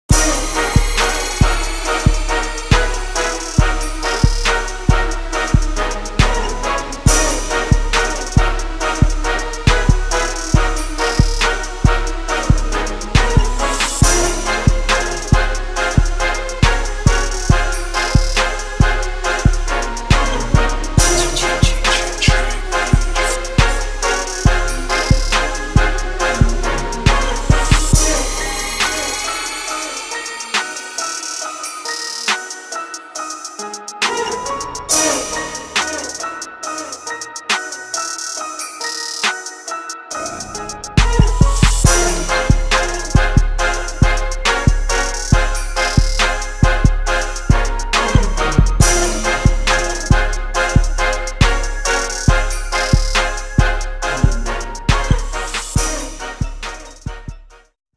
Jordan Scarlett St.Thomas Aquinas RB #22
University of Miami commit Jordan Scarlett Highlights from the 7A Regional Finals Florida State Playoffs 2014 vs Royal Palm Beach High School.

Video produced by Tyrone Timmons